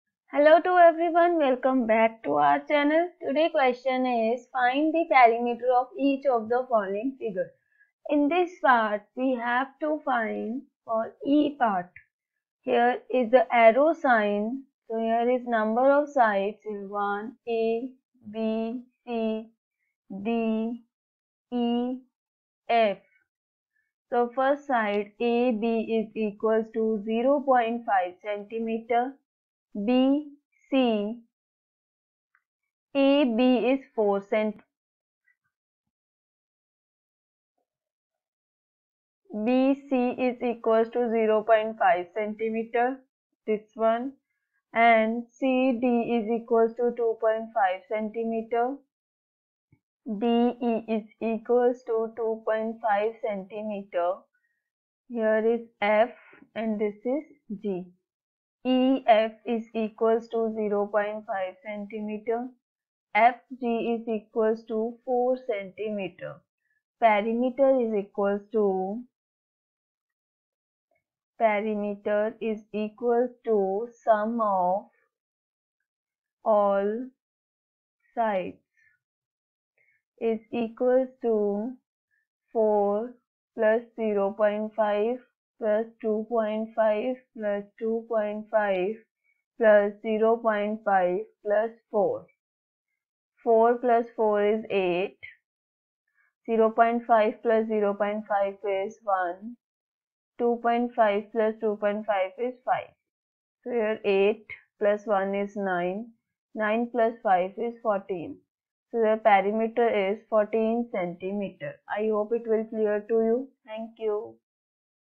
Find the perimeter of each of the following figures: 4cm, 4cm, 0.5cm, 0.5cm, 1cm, 2.5cm, 2.5cm
class 6th
Find the perimeter of each of the following figures